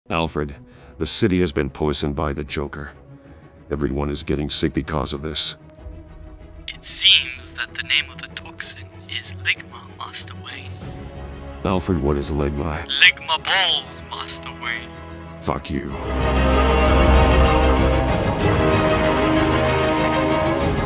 Alfred dies of Ligma
Alfred plays a prank on Batman then dies of Ligma.